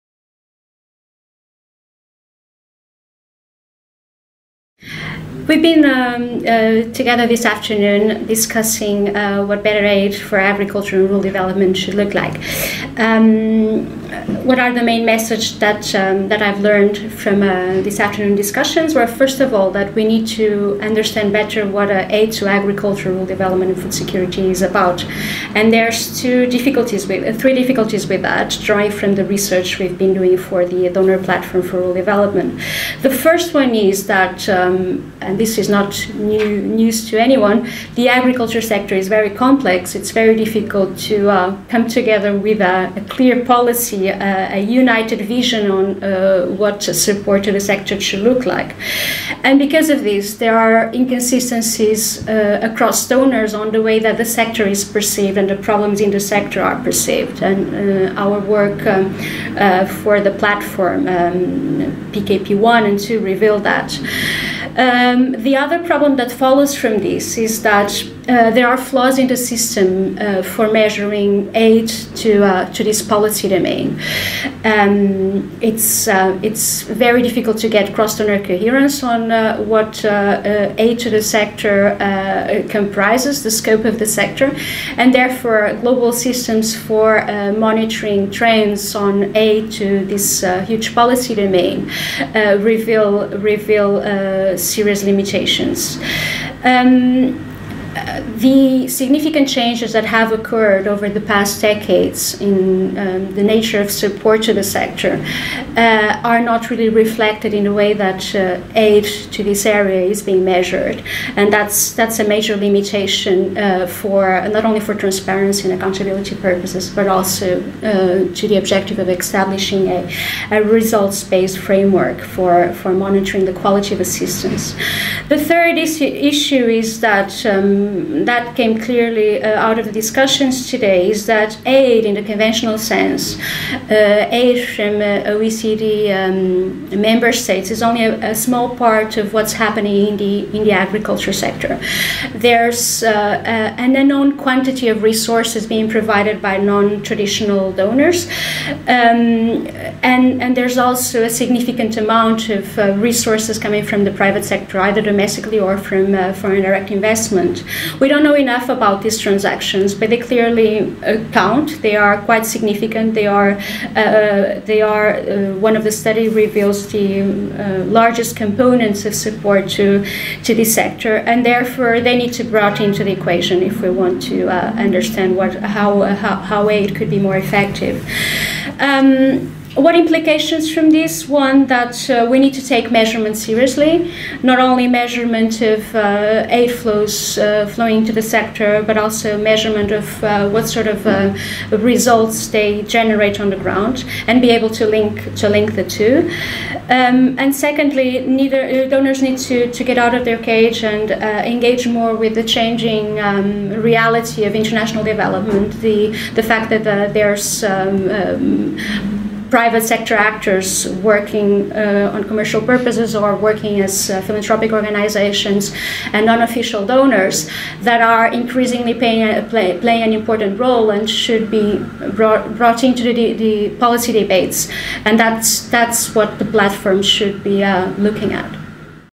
Discussing better aid for ARD__ Lídia Cabral, ODI
Lídia Cabral is a research fellow at ODI and the lead author of Platform Knowledge Piece 2 on aid flows in ARD.

In this video, Lídia gives her concluding remarks on a meeting held at ODI in London on 1 March to wrap-up the Knowledge Piece Series. She addresses the challenges of clearly defining ARD, of measuring aid for results and of bringing the private sector and non-traditional donors into policy debates.